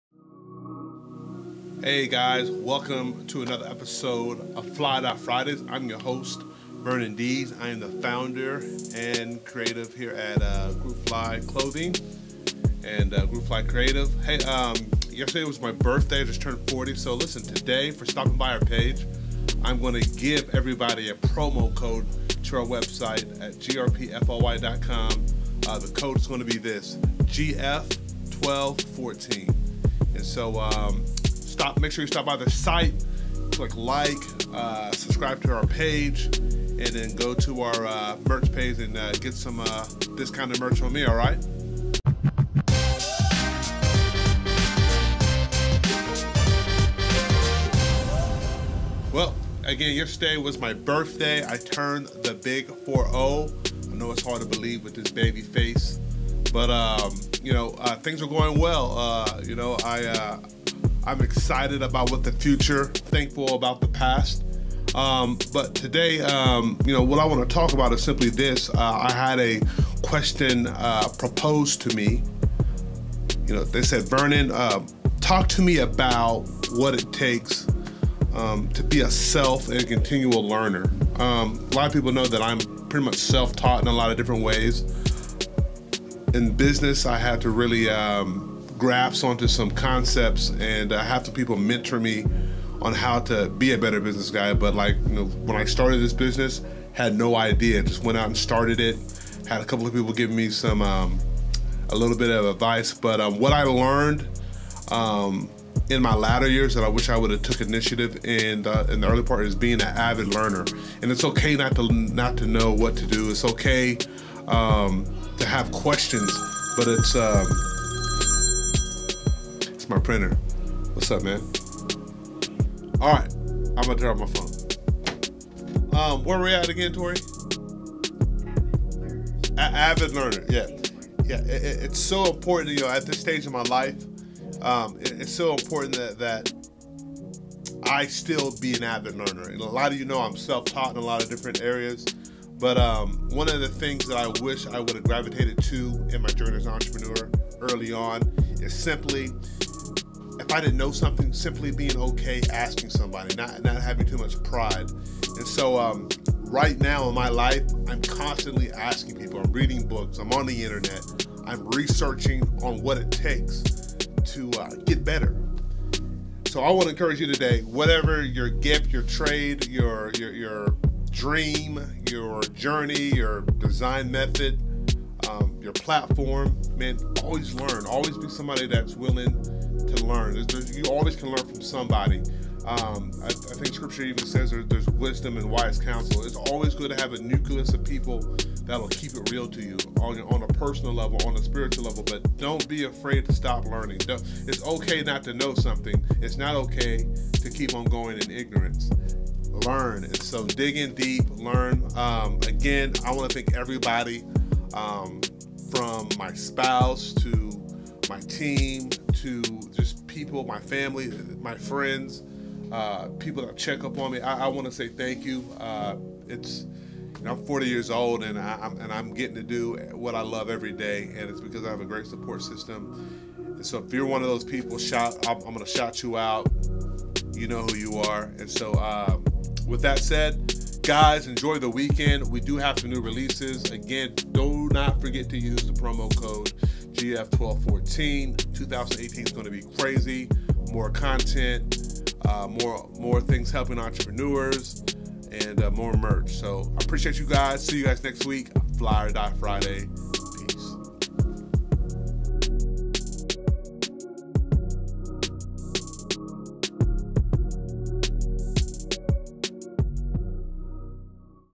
SWALLOW YOUR PRIDE, continuous learning is KEY
--- FLY OR DIE FRIDAYS EPISODE 15 ---

In this installment of FLY OR DIE FRIDAYS Vern shares the importance of continuous learning in business. Start learning as much as you can early on. Stop being prideful and ASK QUESTIONS.

EPISODE 15 SALE PROMO: GF1214

GRP FLY store hours:
Friday & Saturday 11am-6pm
2719 NW 50th St.